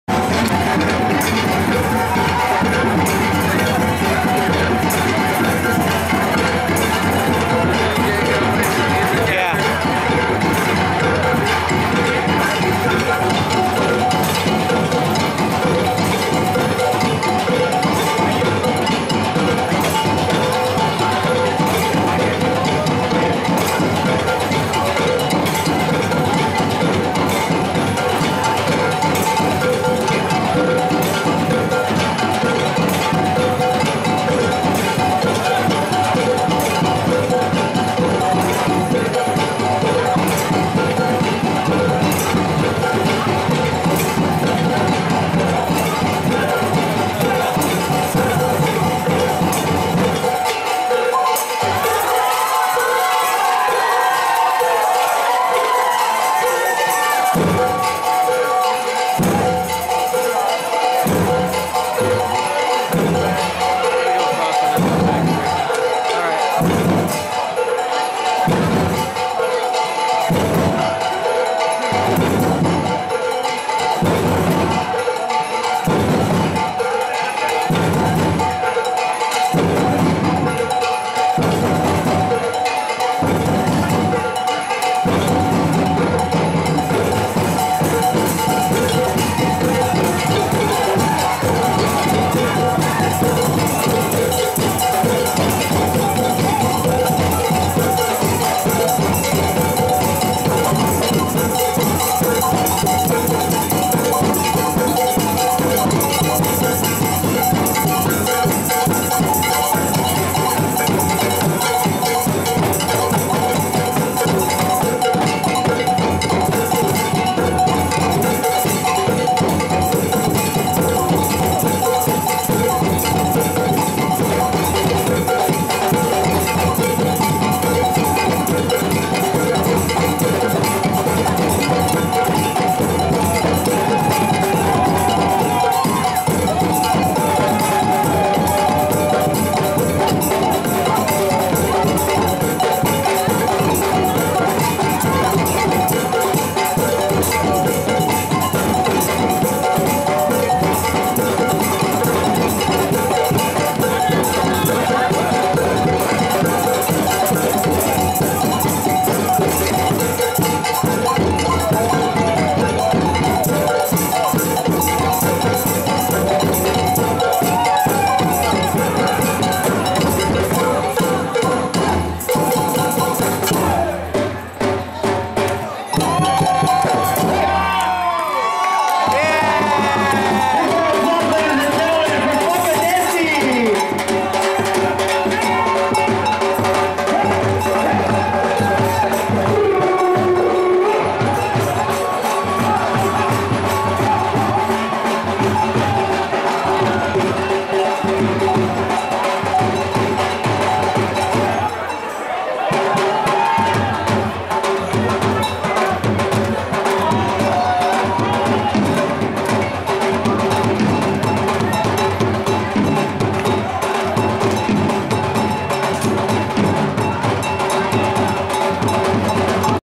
00480 Funkadesi getting crowd really into it Merriott in Indianapolis IN wedding
Awesome Indian wedding.  Thankful to have been a part.  Best food I have ever had...

LOVE FUNKADESI from Chicago